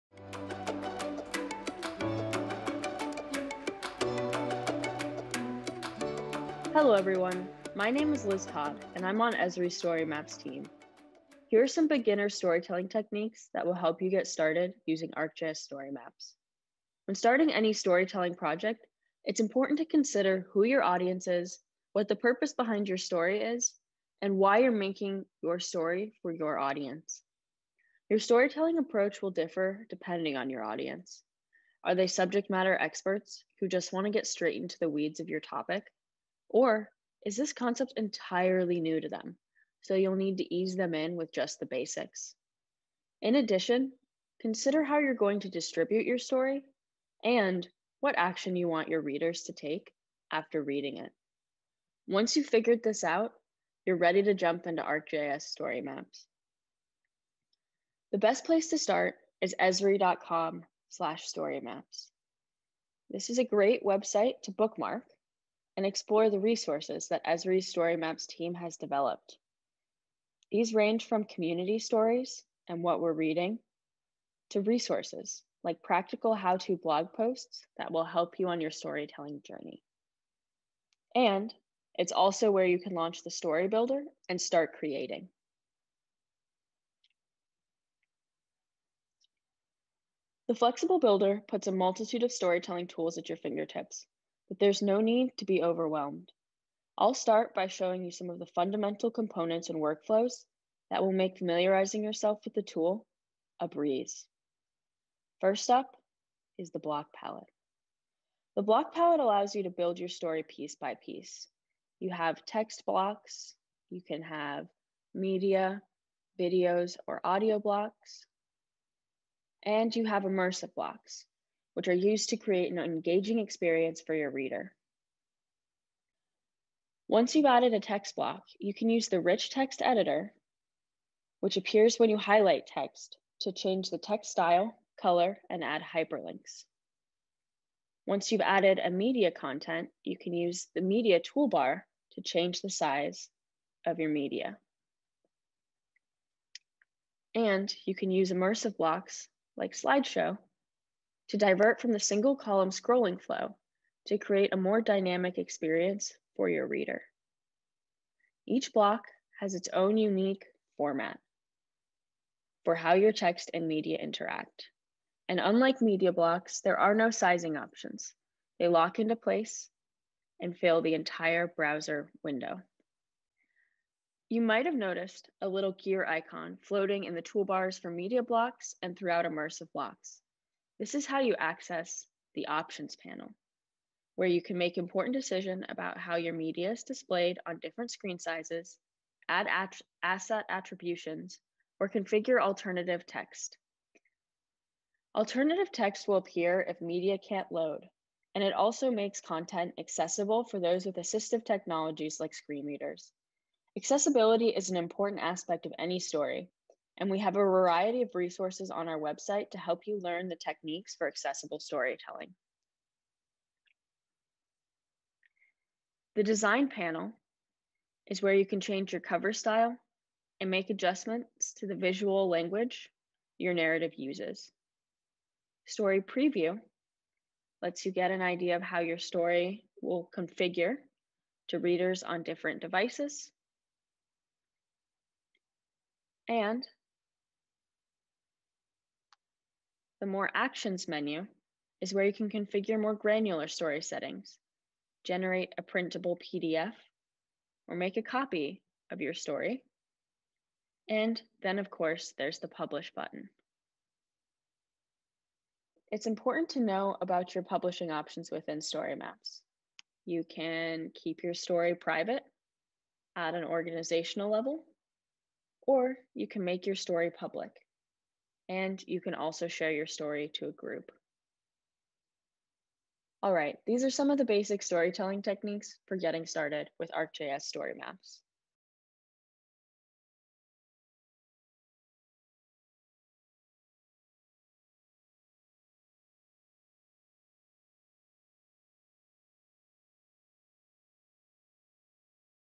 Beginner Storytelling Techniques
Check out these beginner storytelling tips as you get started using ArcGIS StoryMaps.